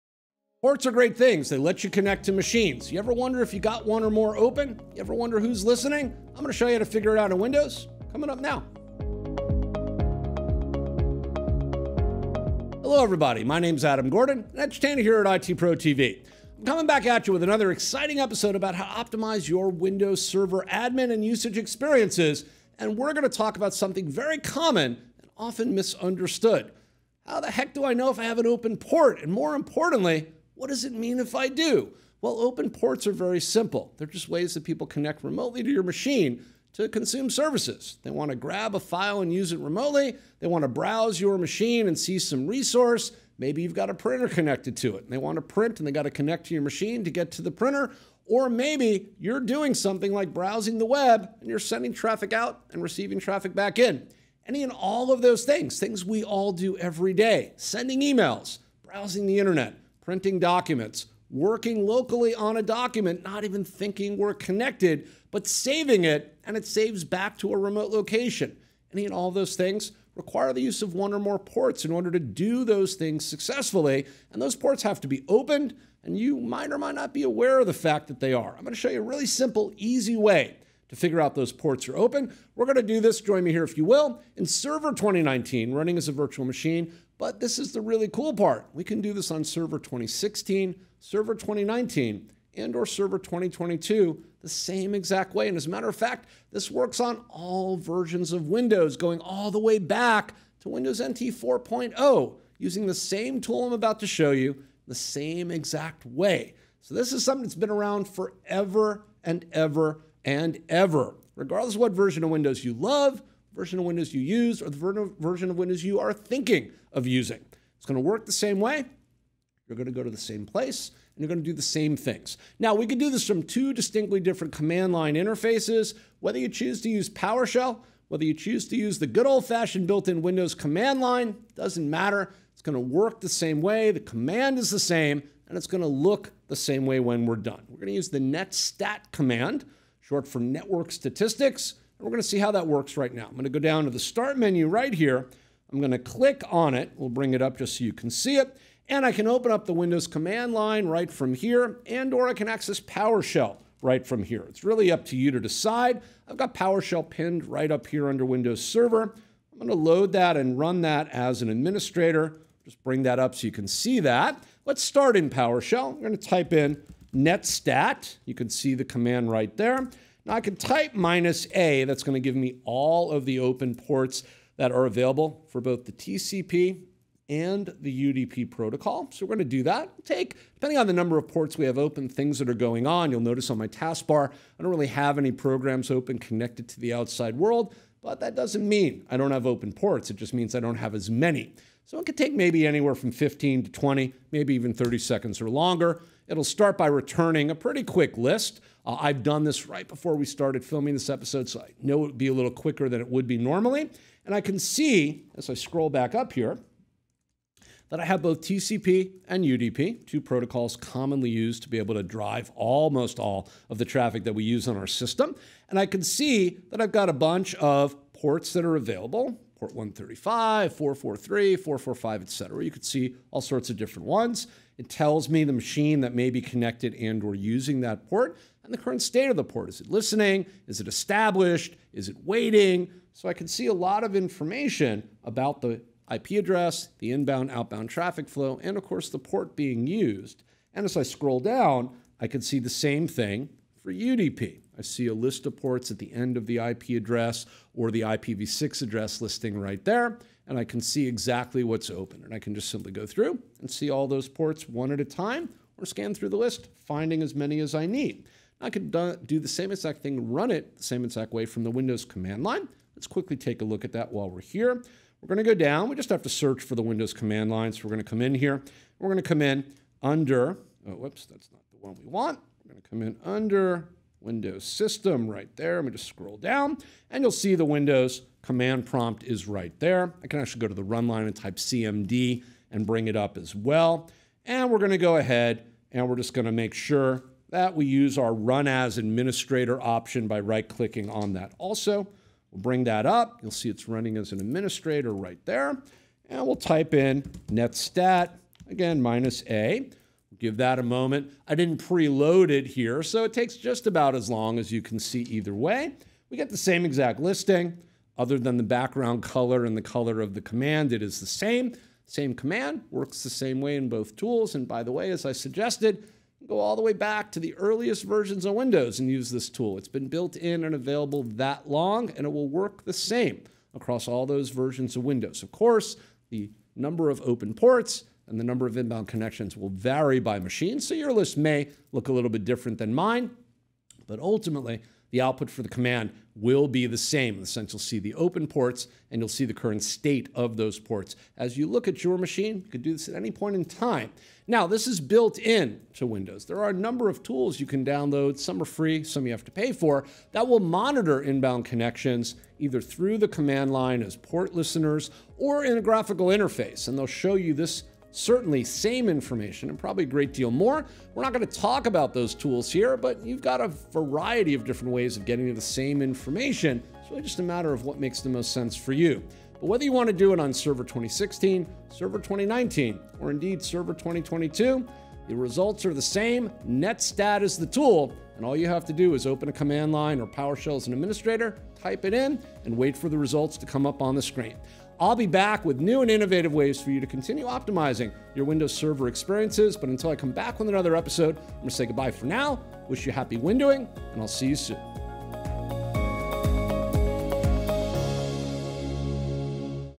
How to Check if a Port is Open on Windows Server (2016, 2019, 2022)
Do you need to check for open ports in Windows Server? Join Adam in this video to learn how to check if a port is open on your computer using two different methods; PowerShell and the Windows Command Line. These methods are the same in Windows Server 2016, 2019, and 2022.

00:00 Intro
02:25 Using PowerShell
04:23 Using Windows Command Line